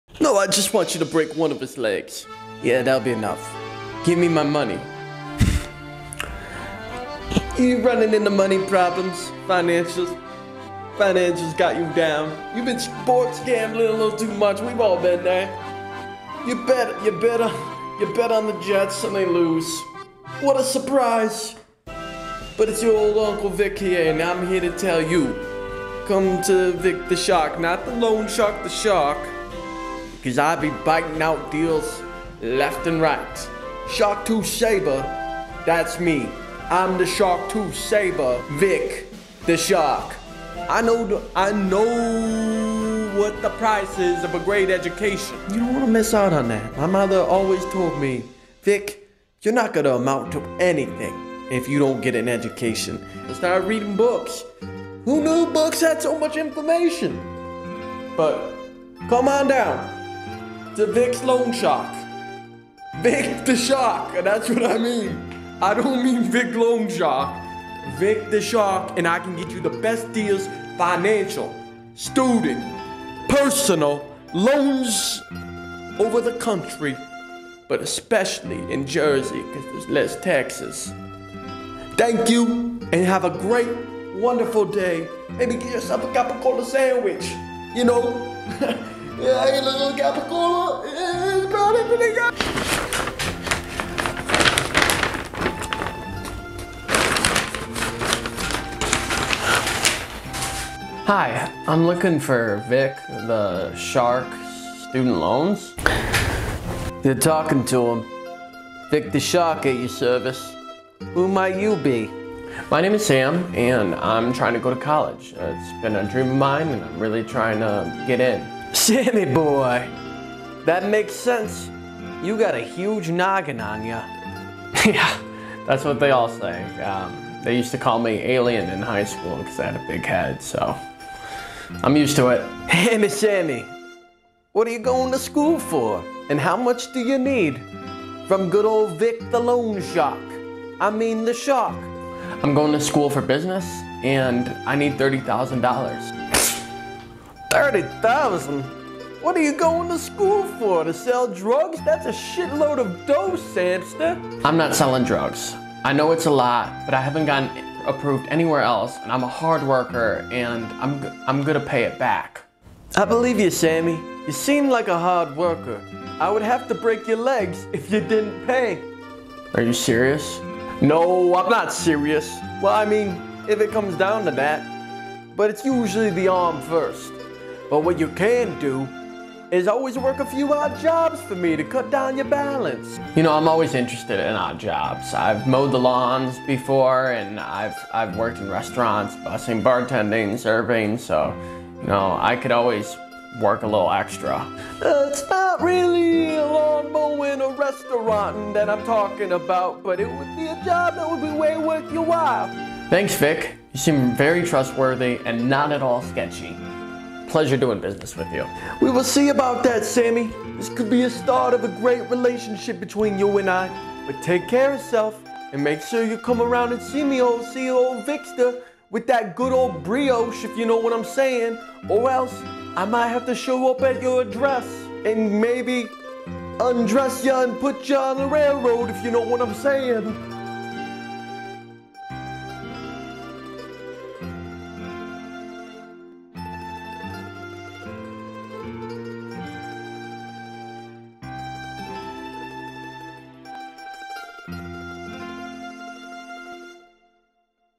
I get a student loan from a loan shark…(I’m screwed)
I have to take out a student loan and I think the guy I got a loan from might be in the mob, loan shark, or both. Watch me get an offer I can’t refuse in this funny interaction between me and this loan Shark…